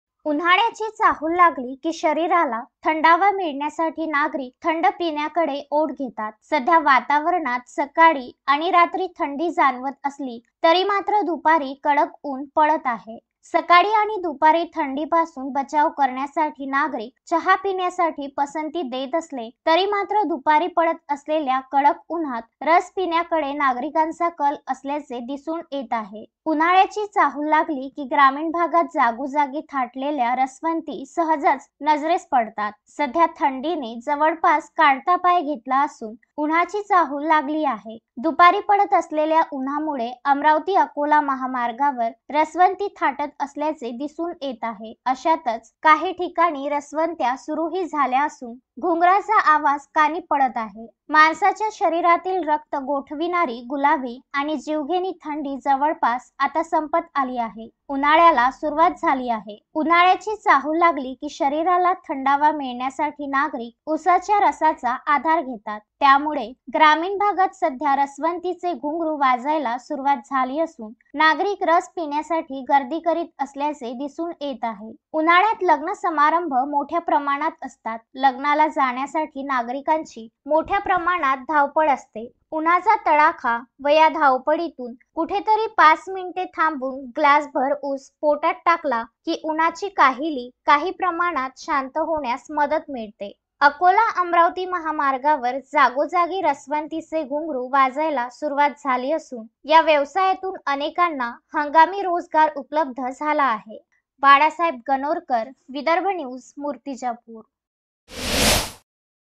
Murtijapur | रसवंतीच्या घुंगरांचा मंजूळ नाद कानावर
उन्हाळ्याची चाहूल लागताच रसवंतीगृहे सुरू
उन्हाळी काहिली शांत होण्यास मदत